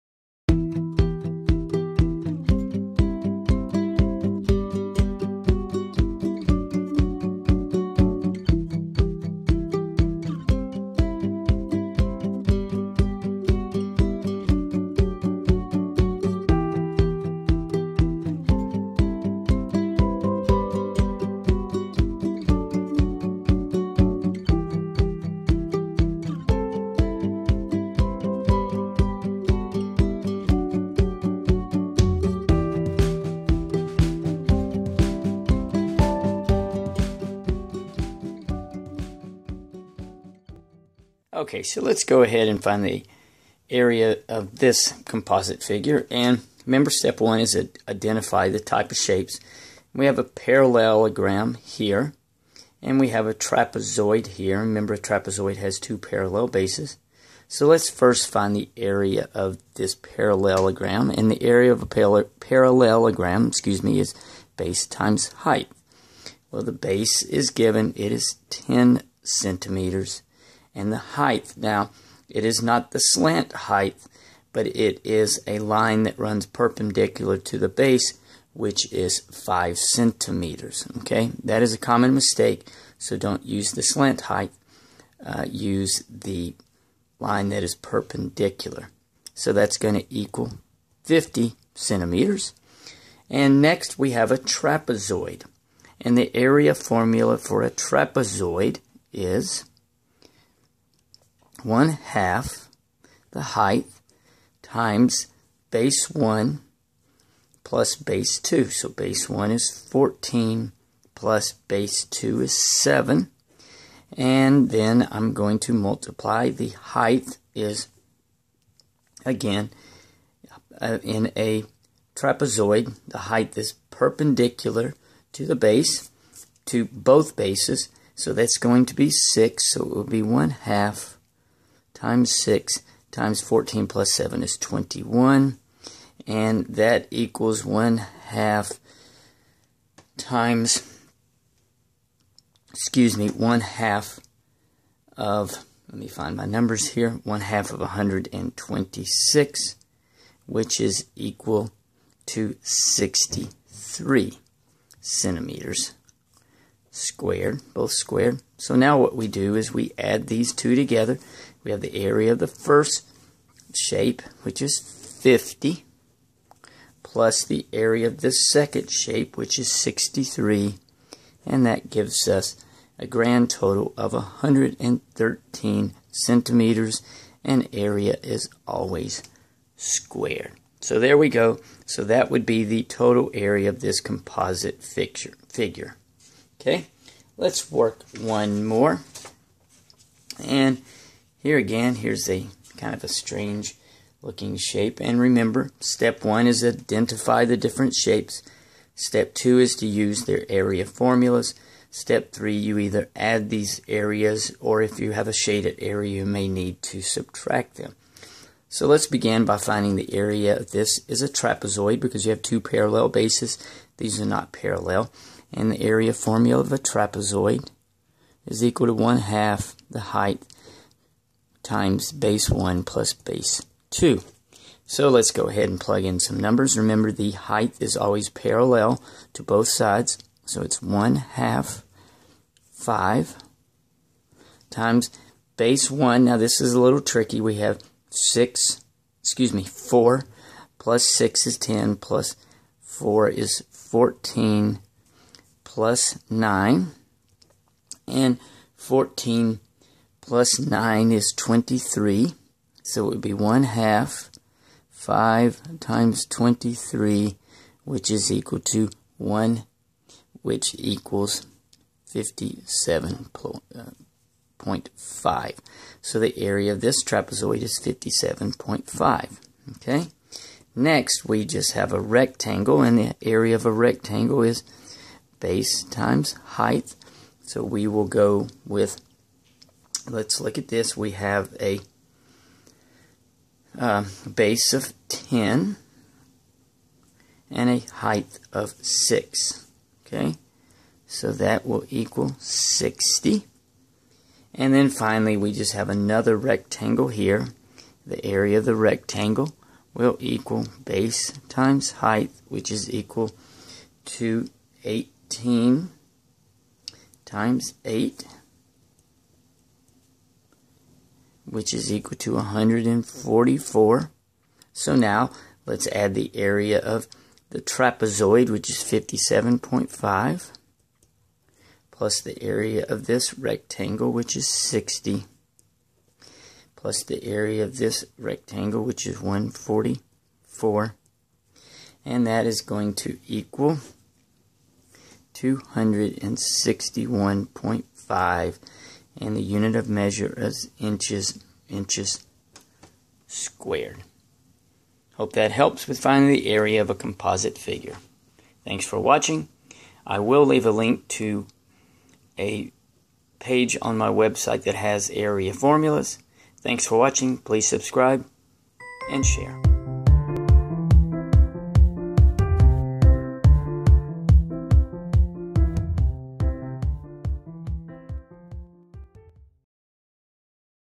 Composite Figures-Finding the Area-Middle School Math
In this video, you will learn how to find the area of composite figures. The examples are appropriate for 7th grade Math, or an introductory high school Geometry class. The height is given in each example.

A composite figure is a geometric shape composed of more than one shape.

Transcript